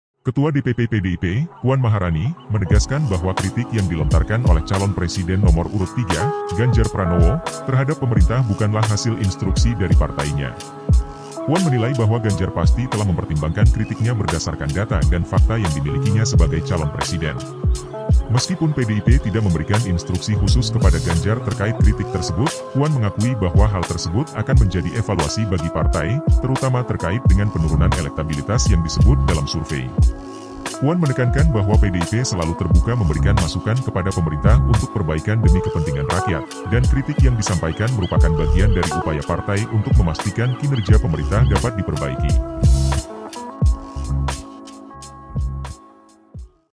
PUAN : KRITIK GANJAR SOAL PENEGAKAN HUKUM BUKAN INSTRUKSI PARTAI
Ketua DPP PDIP, Puan Maharani, menegaskan bahwa kritik yang dilontarkan oleh calon presiden nomor urut 3, Ganjar Pranowo, terhadap pemerintah bukanlah hasil instruksi dari partainya. Puan menilai bahwa Ganjar pasti telah mempertimbangkan kritiknya berdasarkan data dan fakta yang dimilikinya sebagai calon presiden. Meskipun PDIP tidak memberikan instruksi khusus kepada Ganjar terkait kritik tersebut, Puan mengakui bahwa hal tersebut akan menjadi evaluasi bagi partai, terutama terkait dengan penurunan elektabilitas yang disebut dalam survei. Puan menekankan bahwa PDIP selalu terbuka memberikan masukan kepada pemerintah untuk perbaikan demi kepentingan rakyat, dan kritik yang disampaikan merupakan bagian dari upaya partai untuk memastikan kinerja pemerintah dapat diperbaiki.